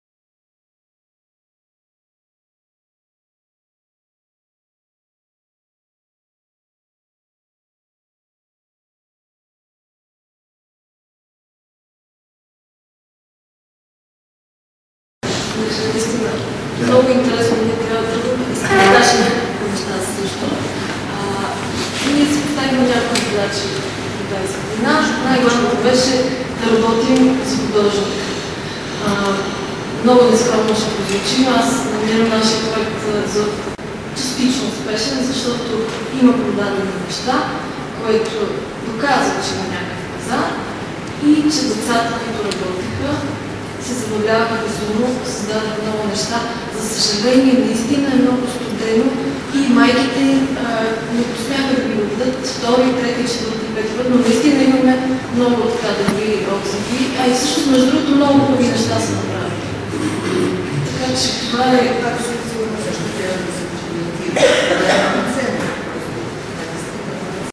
Изказване на Олимпия Николова
Изказвания на участниците в ДИСКУСИЯ по повод на проекта „ИДЕЯ ЗА ДОМ. Съюзът на българските
художници 80 години по-късно"

Ръководството на Съюза на българските художници  организира дискусия по повод на проекта „ИДЕЯ ЗА ДОМ. Съюзът на българските художници 80 години по-късно".

Поканени бяха участниците в изложбите по проекта, художници и студенти,изкуствоведи, журналисти - всички, които се интересуват от изкуство и се чувстват ангажирани с бъдещето на организацията.
Всеки присъстващ имаше възможност да вземе отношение към повдигнатите въпроси.

Дискусията се проведе на 13 декември 2012 г., четвъртък, от 16 часа,в залата на четвъртия етаж на „Шипка" 6.

Видеозапис и Монтаж - Юлиан Митев.